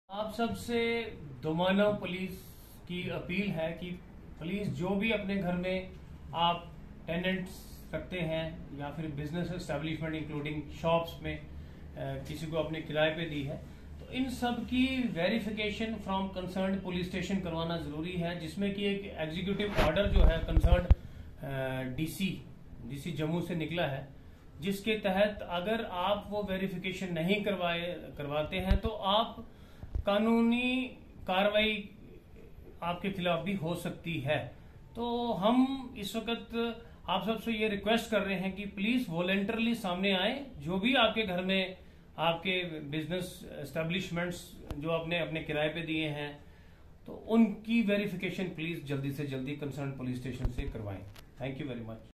एसडीपीओ दोमाना सुनील सिंह ने लोगों से अपने किरायेदारों की जानकारी देने
एसडीपीओ दोमाना सुनील सिंह ने लोगों से अपने किरायेदारों की जानकारी देने की अपील की।

 रिपोर्ट:-सुनील शर्मा